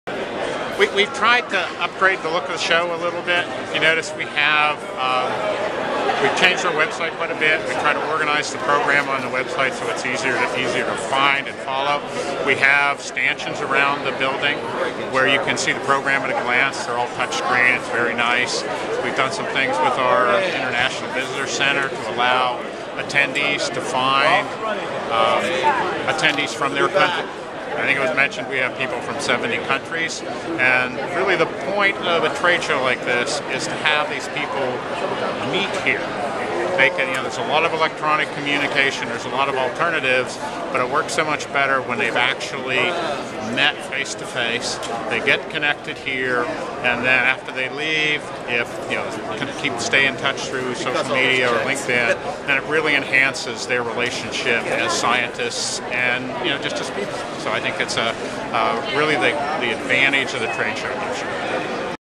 Pittcon 2012 President Jon Peace
Pittcon 2012 President Jon Peace speaking about the value of Pittcon to the Analytic Science community